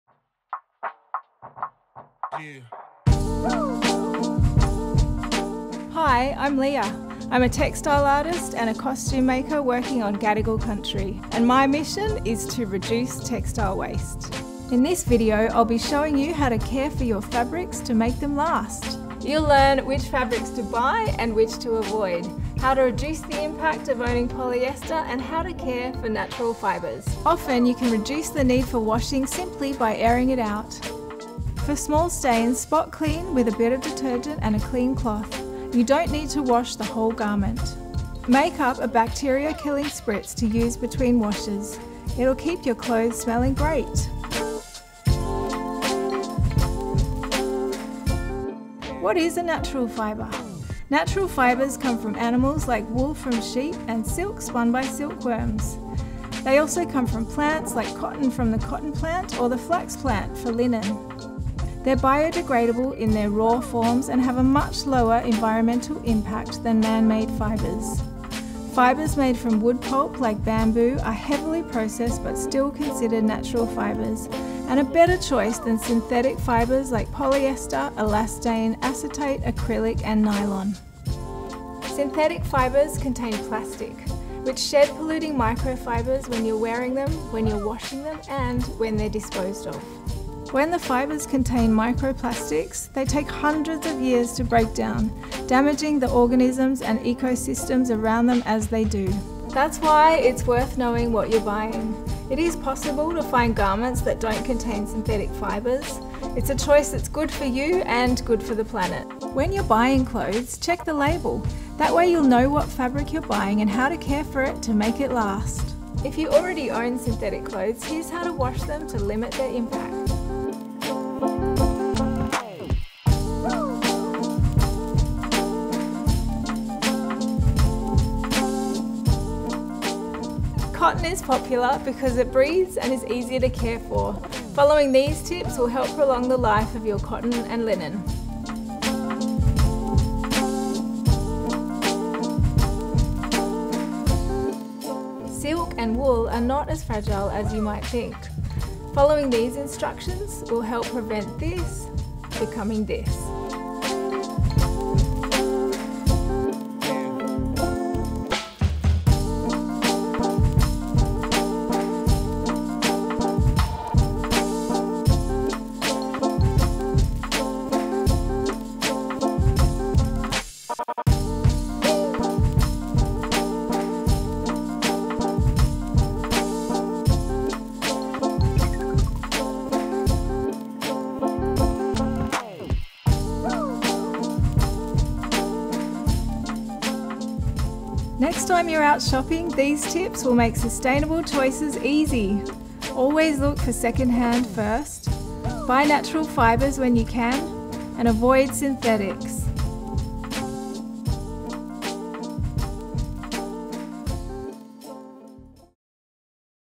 How to reduce textile waste Fabric choice and care
Leah Giblin, creator of clothing label Day Keeper, covers which fabrics to buy and avoid, how to reduce the impact of polyester and caring for natural fibres.